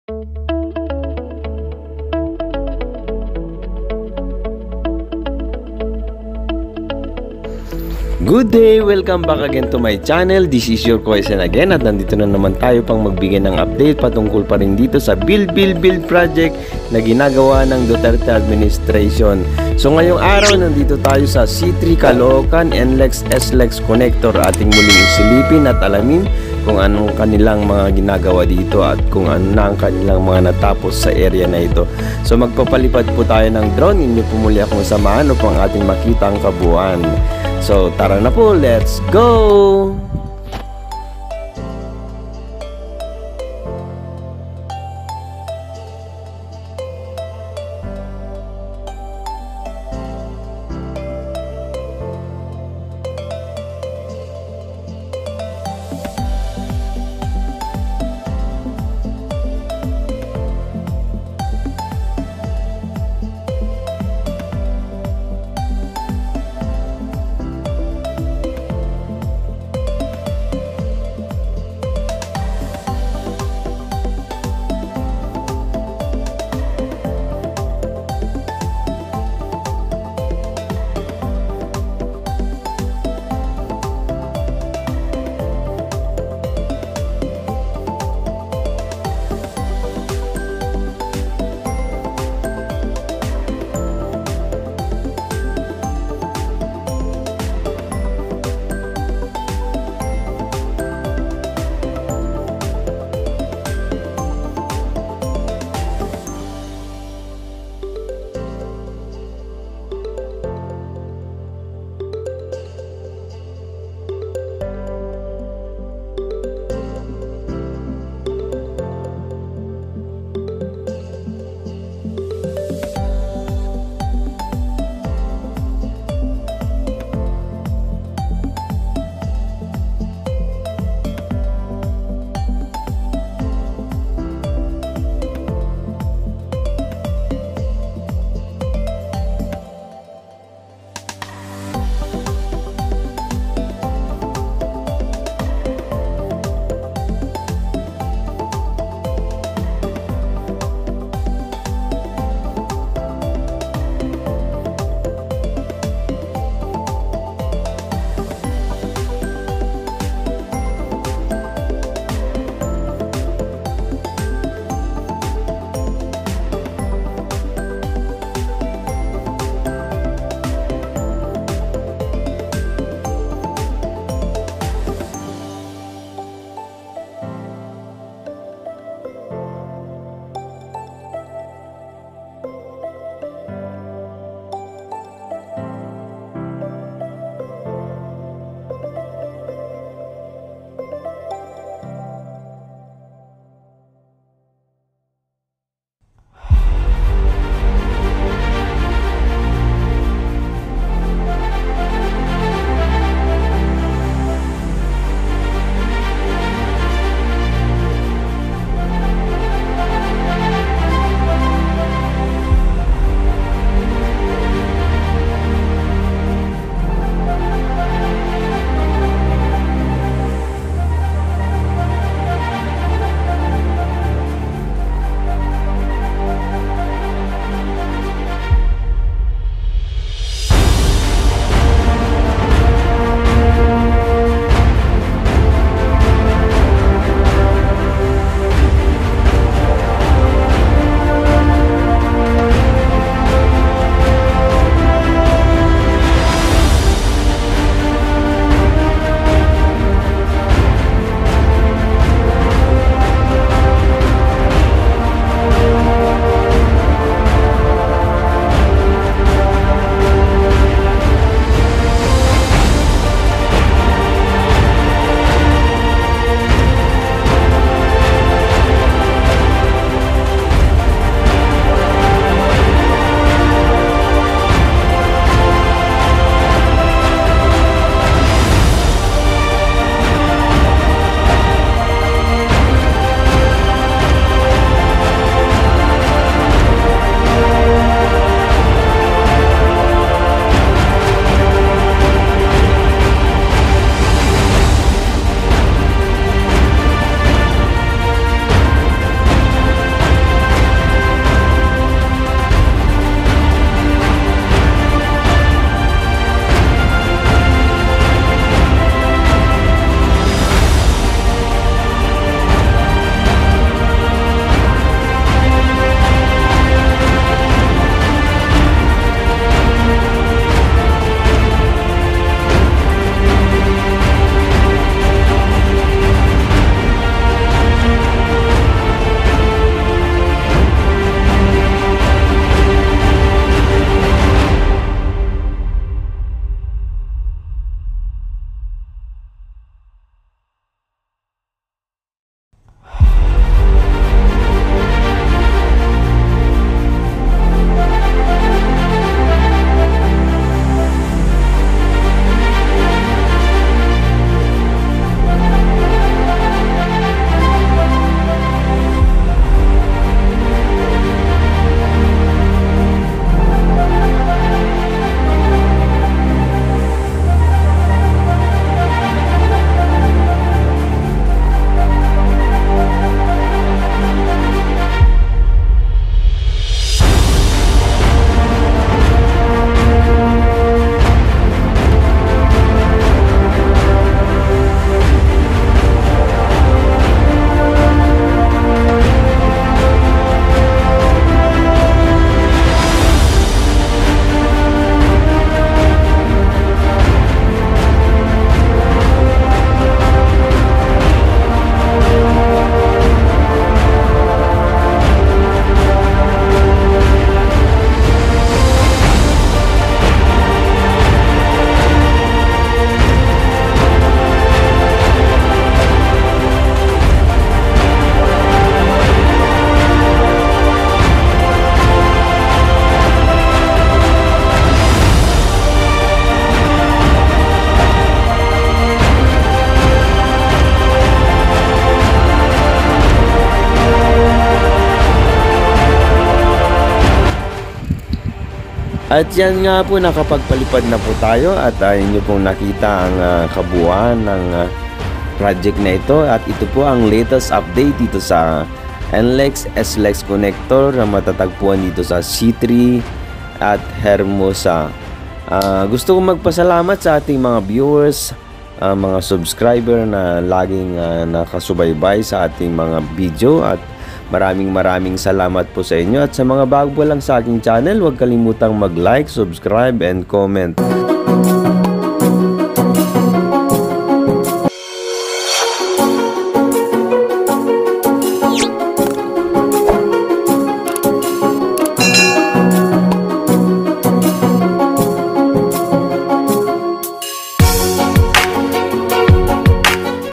NLEX SLEX CONNECTOR UPDATE JAN. 28, 2022 | C3 ROAD CALOOCAN | HERMOSA | build build build
NLEX–SLEX CONNECTOR ROAD PROJECT

Project Description:

An 8 – kilometer all elevated 4-lane toll expressway extending the NLEX southward from the end of Segment 10 in C3 Road Caloocan City to PUP Sta. Mesa, Manila and connecting to the Skyway Stage 3, and mostly traversing the PNR rail track. The project includes two (2) interchanges located at C3 Road/5th Avenue, Caloocan and España, Manila.

Project Objectives:

This project aims to:

stimulate development in Manila, Caloocan, Malabon and Navotas, as well as its surrounding areas.
decongest Metro Manila traffic
provide trucks with 24/7 alternative route and resolve truck-ban problems
provide better access to Manila Ports (North Harbor) and Airports (NAIA and Clark)
complete the connection of NLEx with SLEx

Project Benefits:

At the end of construction, the project is expected to:

reduce travel time from SLEX to NLEX from 2 hours to just 20 minutes
reduce travel time from Clark to Calamba from approximately 3 hours to 1 hour and 40 minutes.
benefit at least 35,000 motorists/vehicles per day
improve connectivity between NAIA and Clark airports.